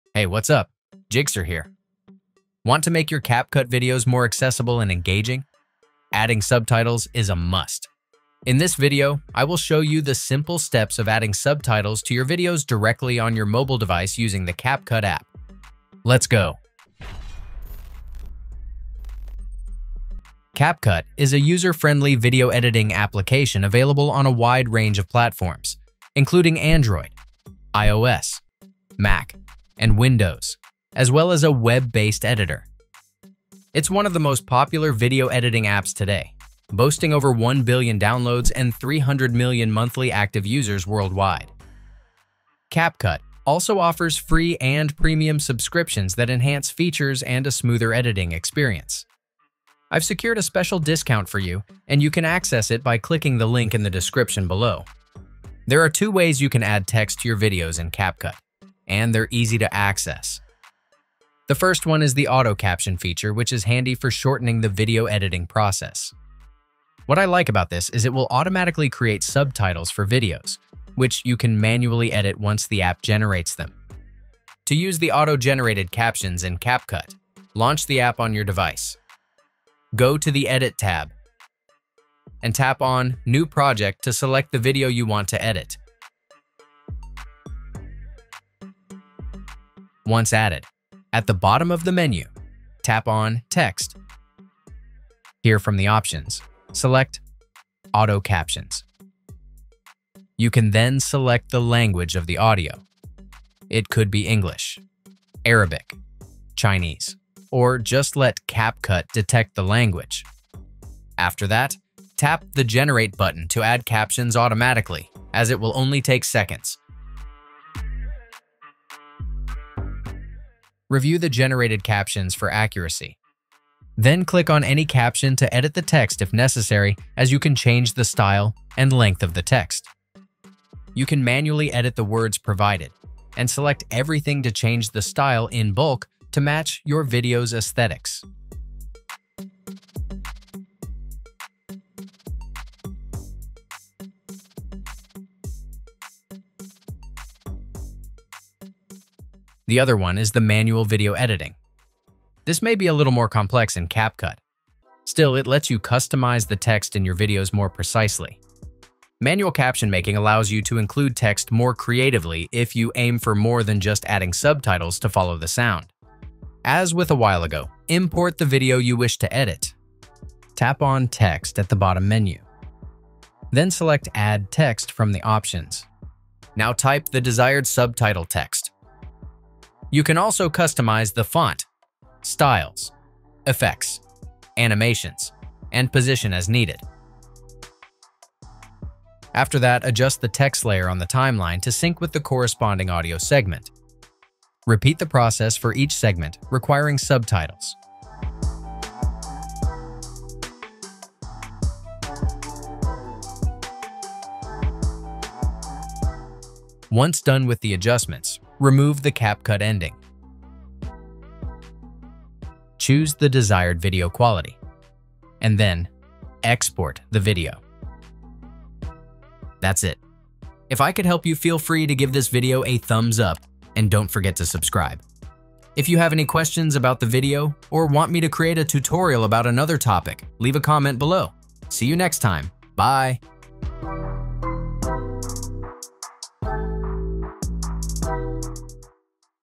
How To Add Subtitles In CapCut! [Mobile Edition]
Want to make your CapCut videos more accessible and engaging? CapCut, a popular video editing app, makes it easy to add subtitles to your mobile videos. Whether you're creating content for social media, educational purposes, or simply enhancing accessibility, this guide will show you the simple steps of adding subtitles to your videos directly on your mobile device using the CapCut app. Yours Jigxor

Most important links:
* | Get CapCut Pro

Timestamps:
00:00 | CapCut Subtitle
00:55 | Auto Captions
02:29 | Manually Added Text

Video: 1209